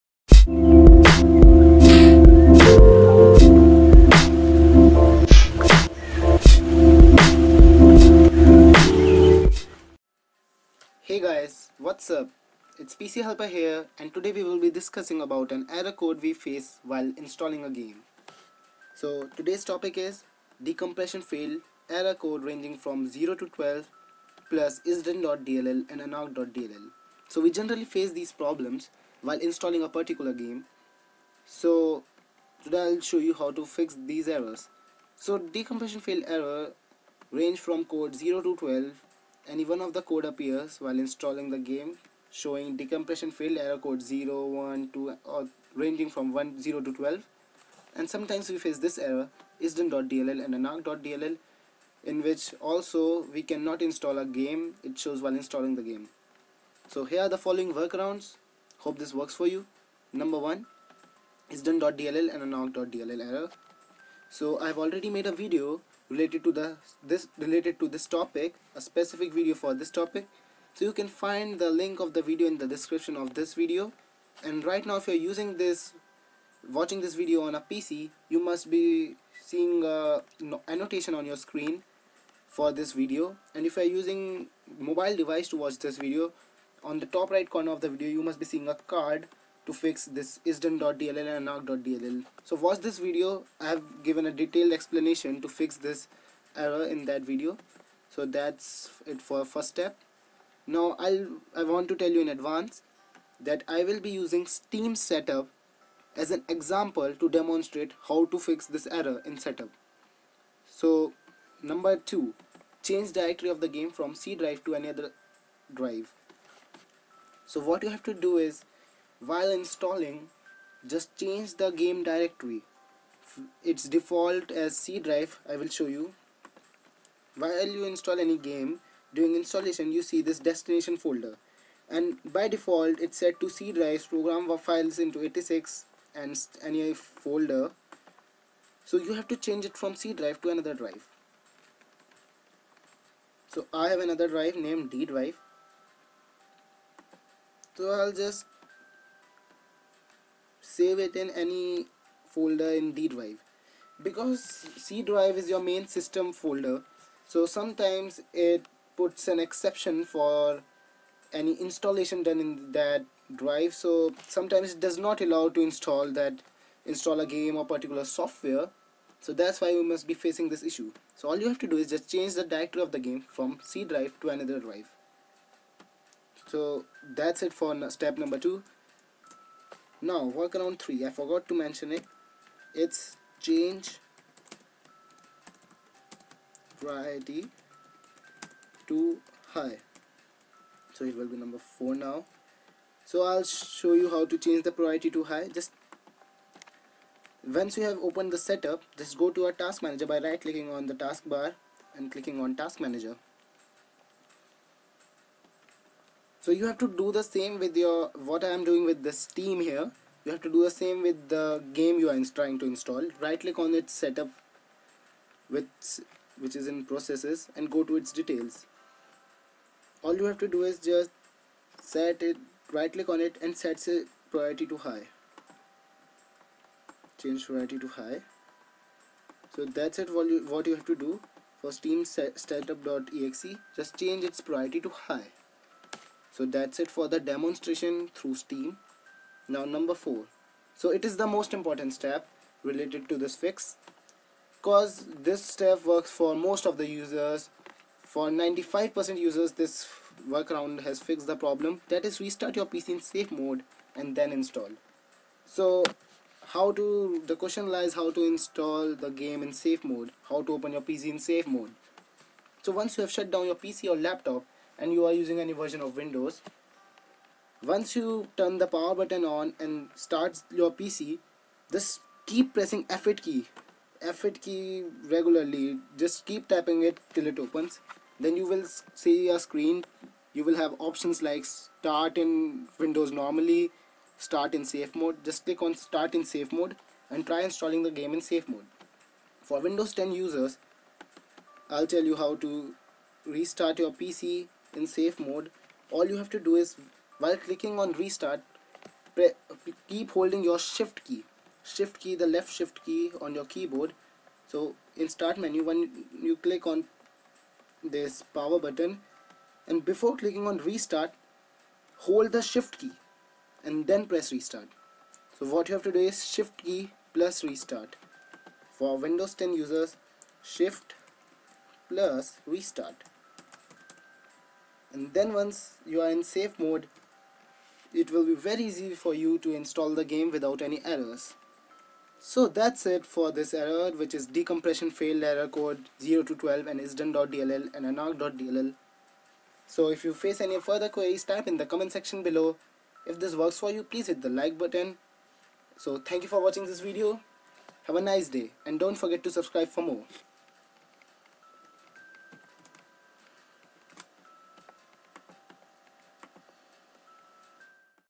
Decompression Failed Error Code (0-12) + ISDone.dll And Unarc.dll For All Games[FIXED]2017
Decompression Failed Error Code (0-12) + ISDone.dll And Unarc.dll
Work Arounds-

2) Change Directory Of The Game From C Drive To Any Other Drive.

3)Change Priority to high

4)Restart Your PC In Safe Mode And Then Install.

  -Press F8 While Starting Your PC

FOR WINDOWS 10

  -Press Shift While Restarting Your PC.[SHIFT+Restart]